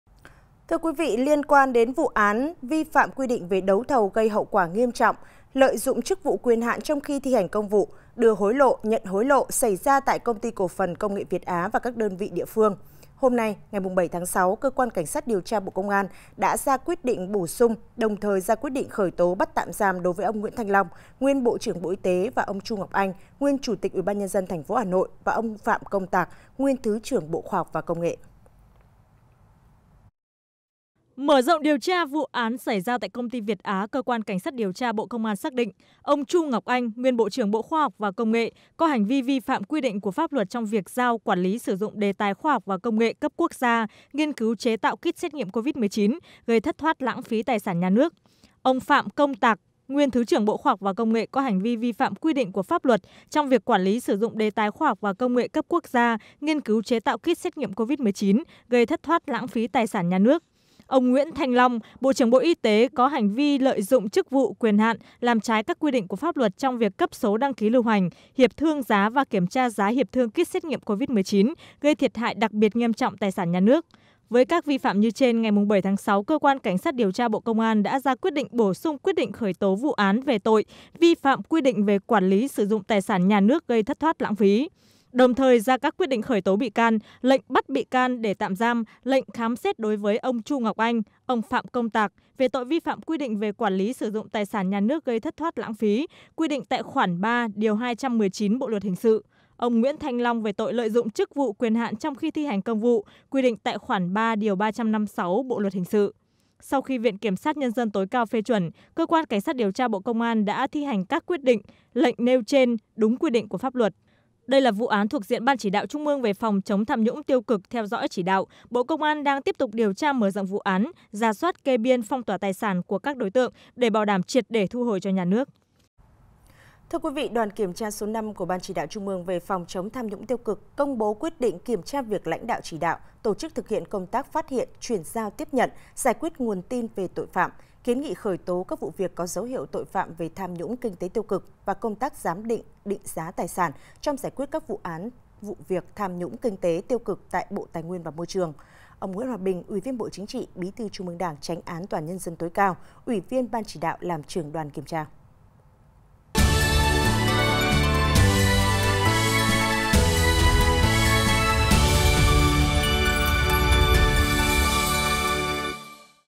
Khởi tố, bắt tạm giam đối với ông Nguyễn Thanh Long và Chu Ngọc Anh
Khởi tố, bắt tạm giam đối với ông Nguyễn Thanh Long và Chu Ngọc Anh | Truyền Hình Nhân Dân
1. Dự Báo Thời Tiết Hôm Nay